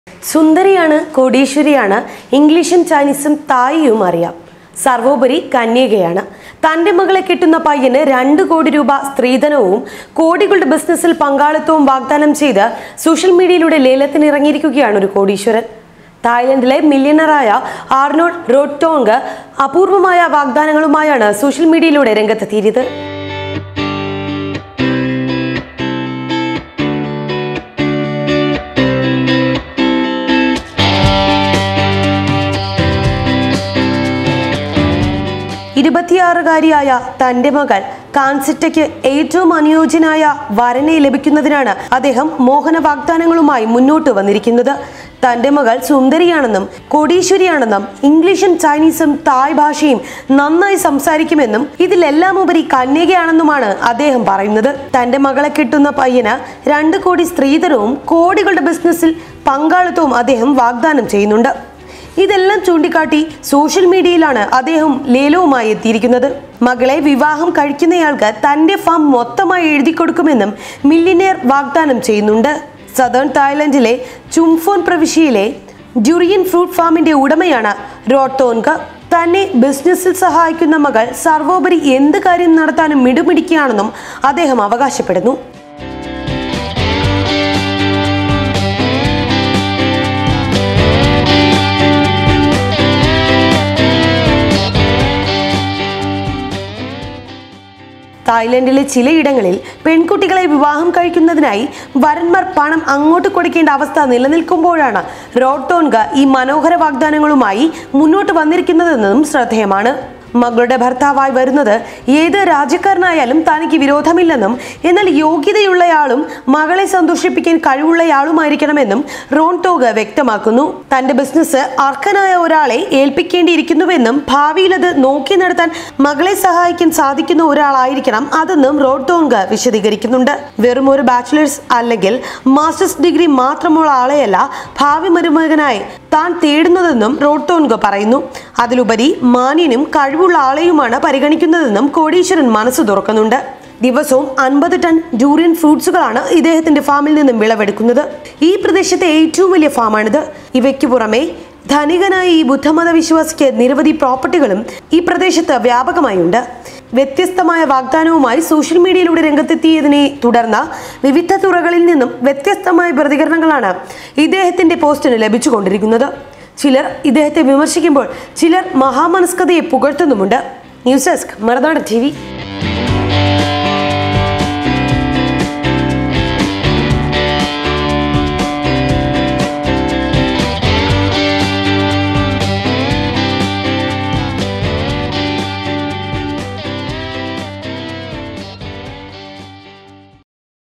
കോടീശ്വരിയായ മകള്‍ക്ക് പയ്യനെ തേടി അച്ഛന്റെ ലേലംവിളി I Thailand millionaire
സുന്ദരിയാണ്...കോടീശ്വരിയാണ്...ഇംഗ്ലീഷും ചൈനീസും തായിയും അറിയാം; സര്‍വോപരി കന്യകയാണ്; തന്റെ മകളെ കെട്ടുന്ന പയ്യന്  രണ്ട് കോടി സ്ത്രീധനവും കോടികളുടെ ബിസിനസില്‍ പങ്കാളിത്തവും വാഗ്ദാനം ചെയ്ത് സോഷ്യല്‍ മീഡിയയിലൂടെ ലേലത്തിനിറങ്ങി കോടീശ്വരന്‍. തായ്ലണ്ടിലെ മില്യണയറായ ആര്‍നോണ്‍ റോഡ്തോന്‍ഗ് അപൂര്‍വമായ വാഗ്ദാനങ്ങളുമായാണ് സോഷ്യല്‍ മീഡിയയിലൂടെ രംഗത്തെത്തിയിരിക്കുന്നത്.